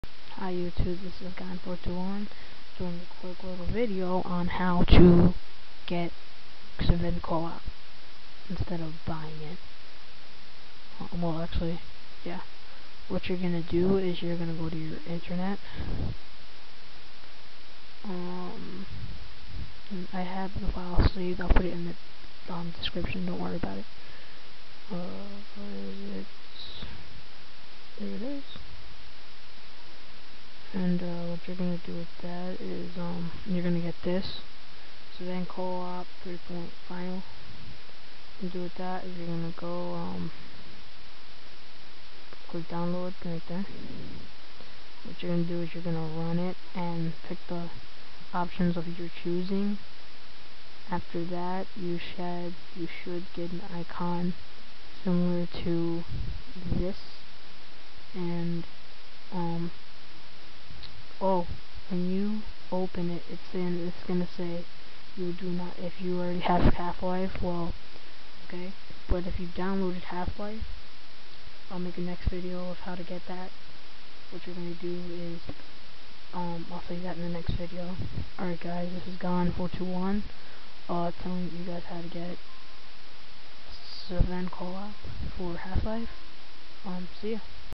How to get Sven Coop for your computer.READ DISCRIPTION
Hey guys 2vids in aday lucky duckies. =D

Like in my other vid i but it in steps
step1:go to this website
step2:download it
step3:save it to YOUR desktop
step4:ill make a new video how to make it work if u downloaded
cs1.6 and it comes with halflife so BE HAPPY thts why im doing this ok.